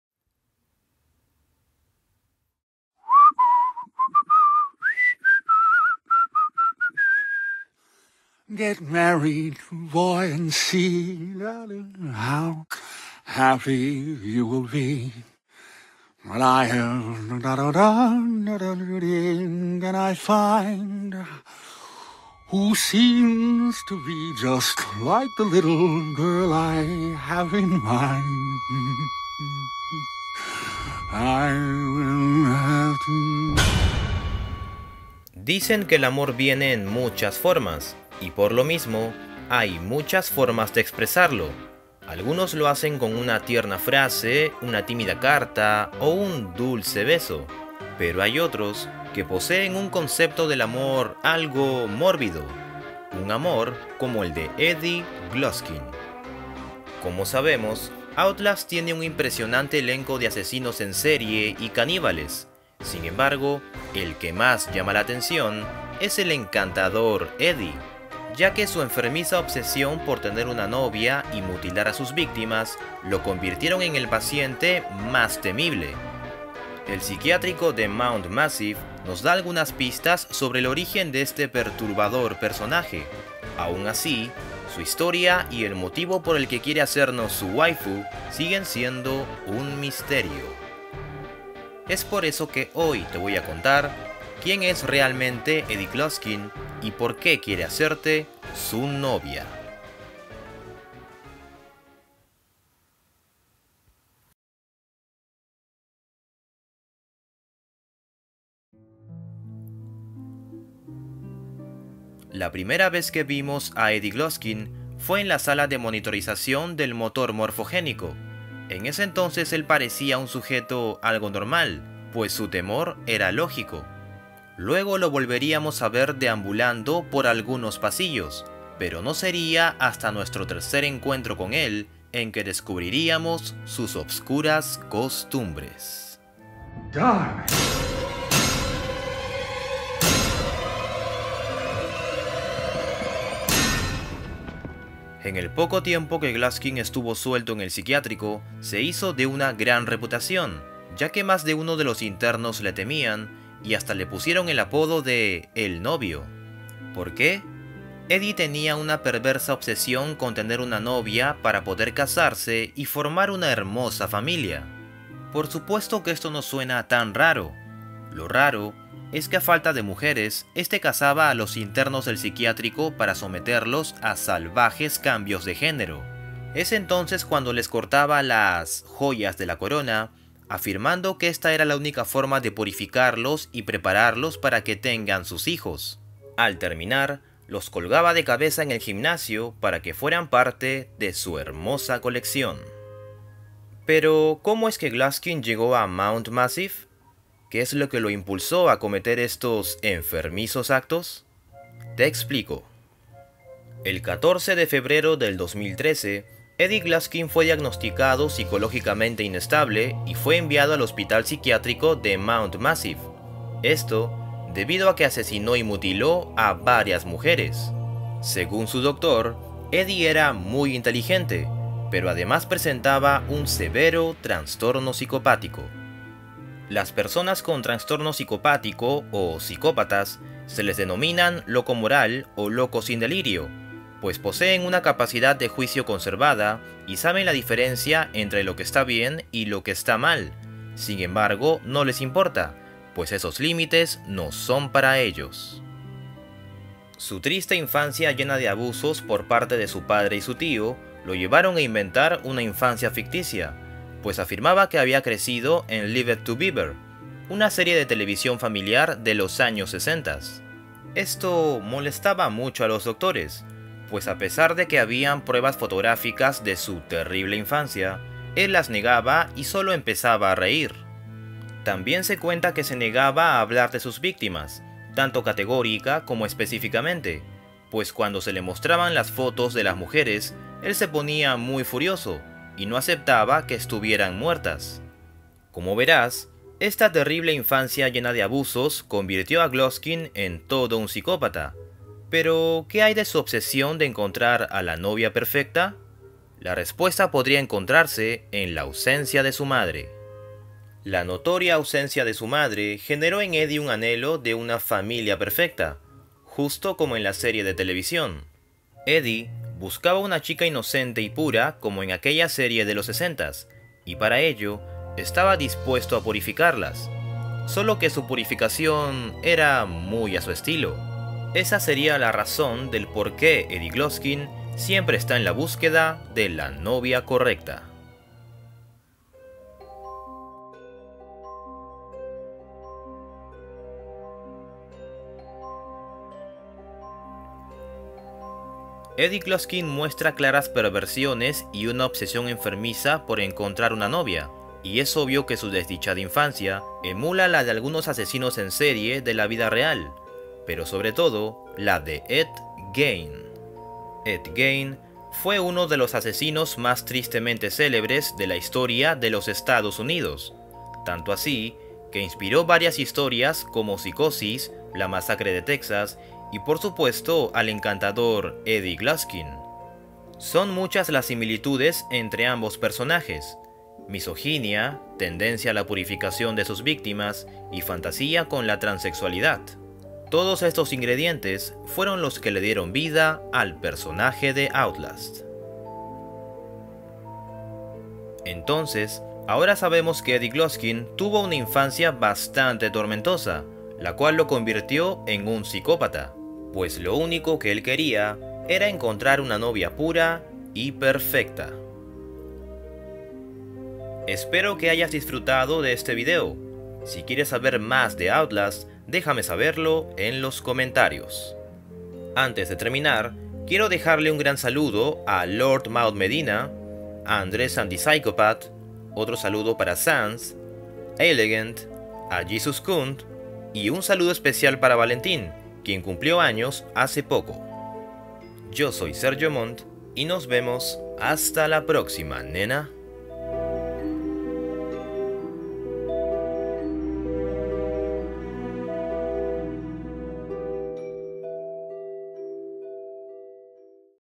¿Por qué Eddie Gluskin quiere hacerte su novia? - Outlast
¿Quién es Eddie Gluskin? ¿Por qué quiere hacerte su novia? Hoy te cuento uno de los misterios de Outlast The whistleblower.